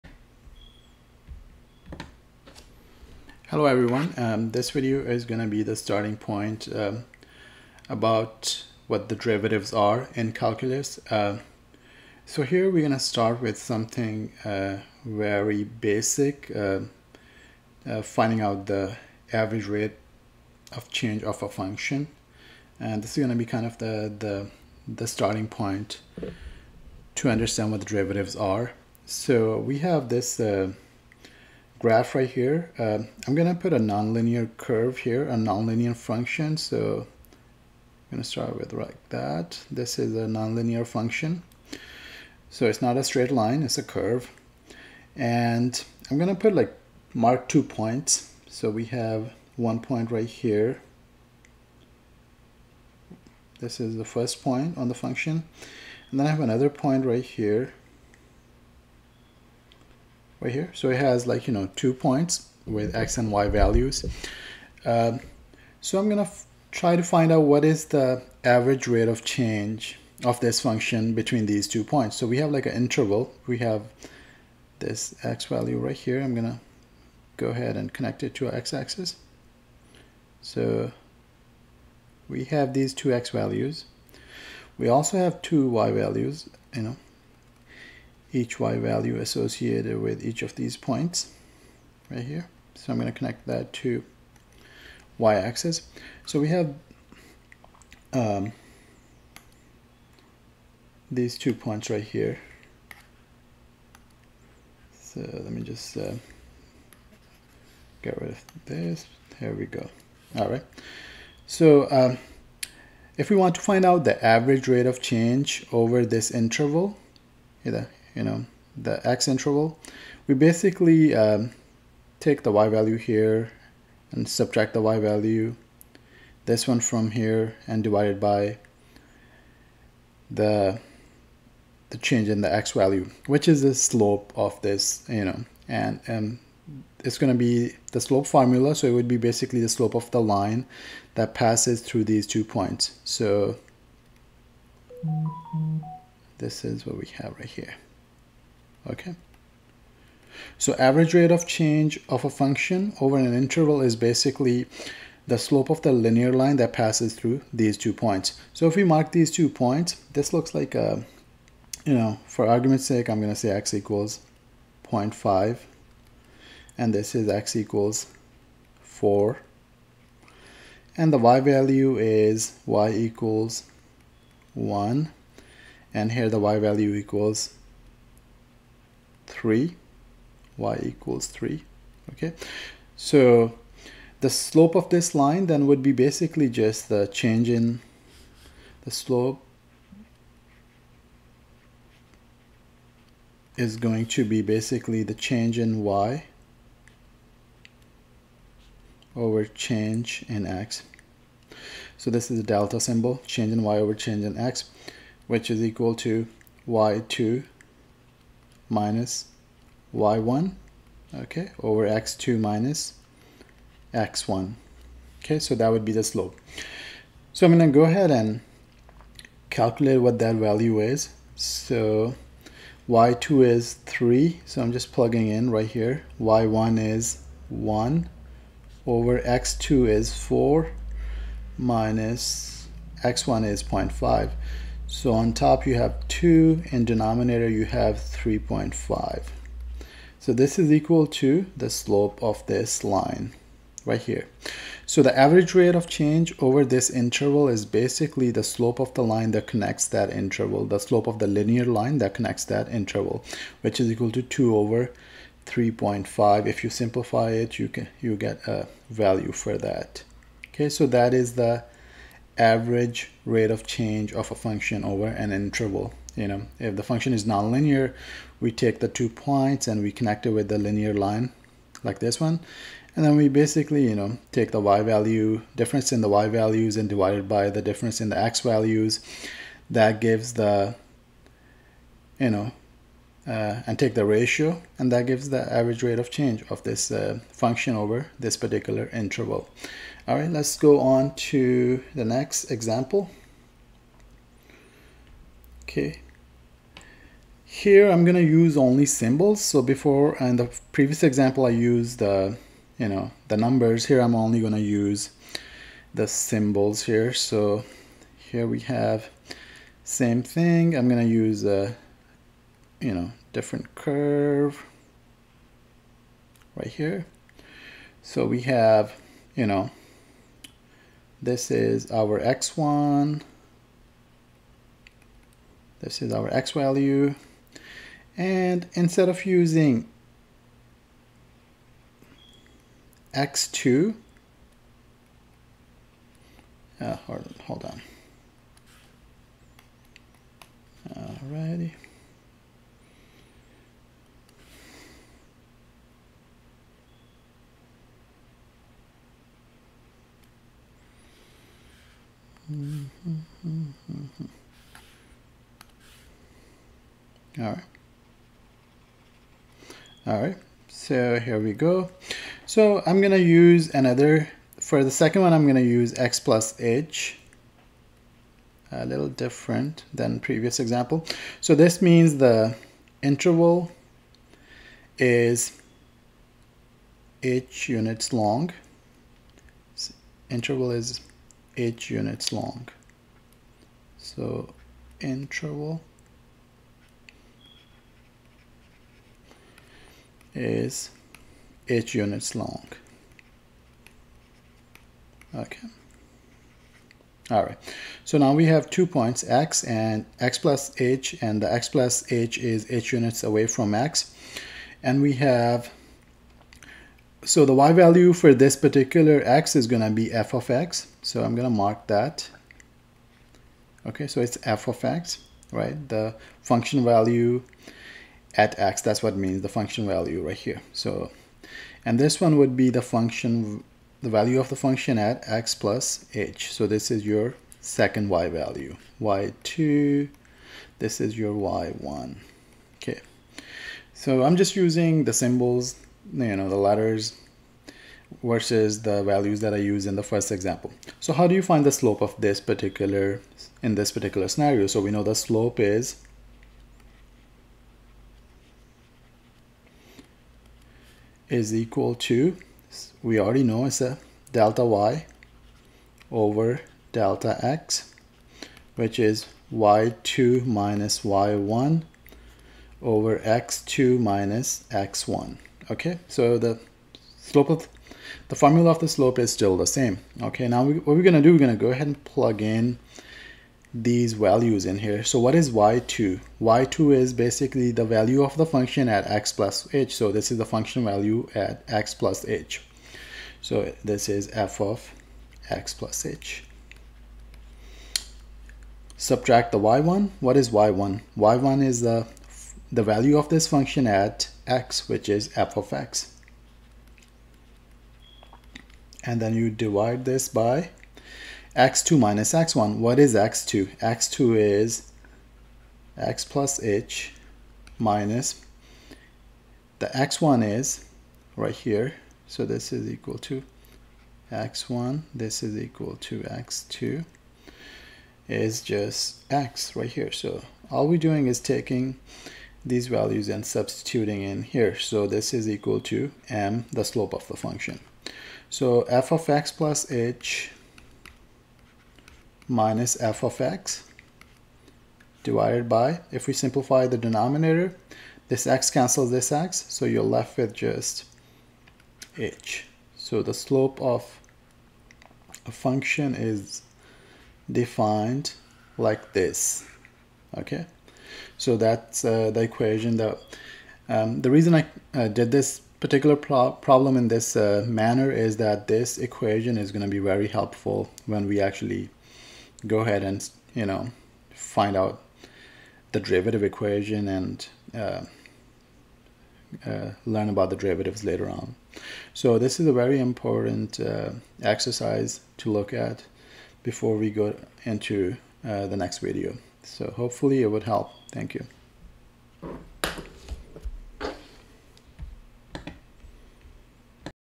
Average rate of change of a function over an interval. derivatives part 1
This video goes over finding an average rate of change of a function over an interval